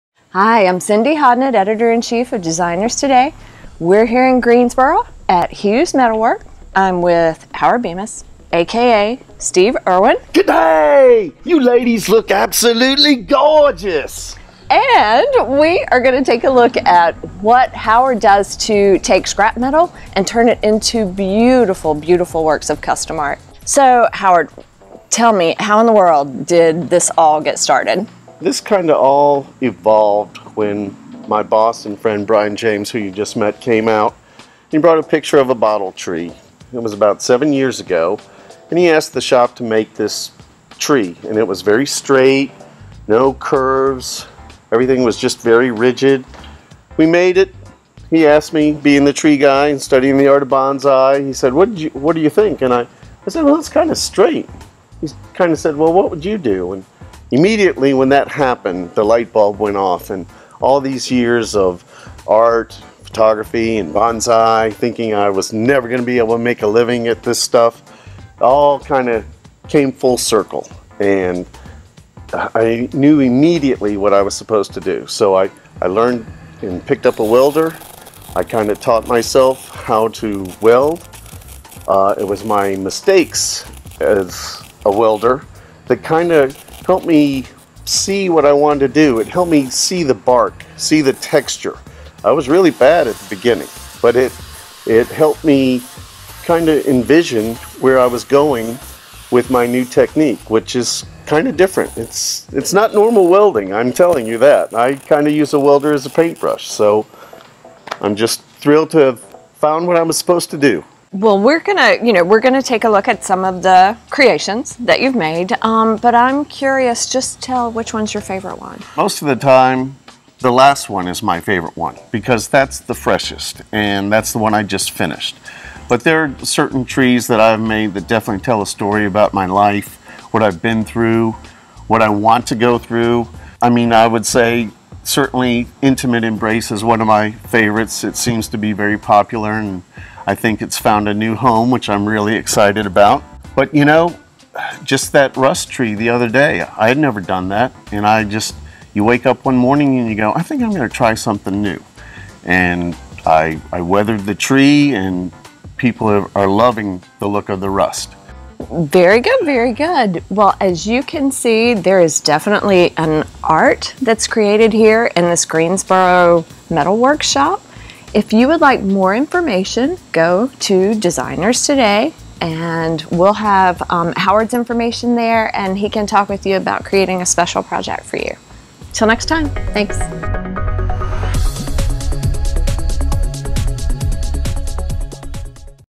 Take a Walk Through the Steel Forest with Howard Bemus
Artisan Spotlight: Howard Bemus creates enchanting forests from metal scraps in a North Carolina metal shop. From the intertwined branches of Intimate Embrace to the newest addition of rust to the product repertoire, Bemus’ Bonsai trees are art and craftsmanship in one.
For the skinny on all the latest in home designers, trends and fashion